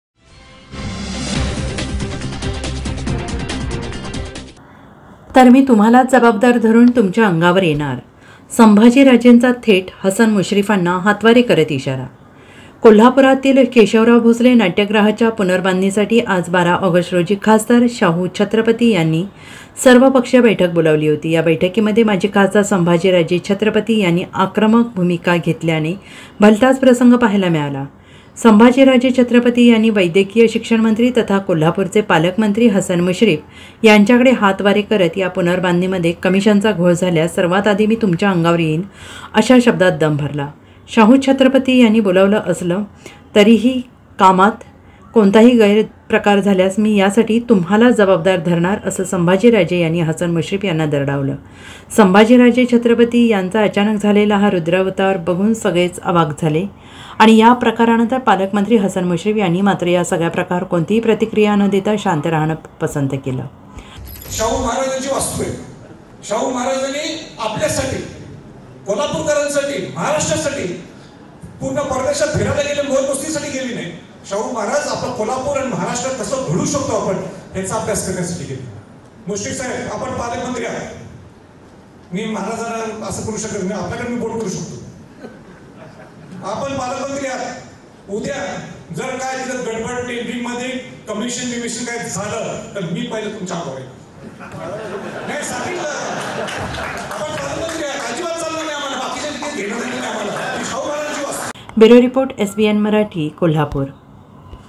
KOLHAPUR | तर मी तुम्हालाच जबाबदार धरून तुमच्या अंगावर येणार!
तर मी तुम्हालाच जबाबदार धरून तुमच्या अंगावर येणार! संभाजीराजेंचा थेट हसन मुश्रीफांना हातवारे करत इशारा.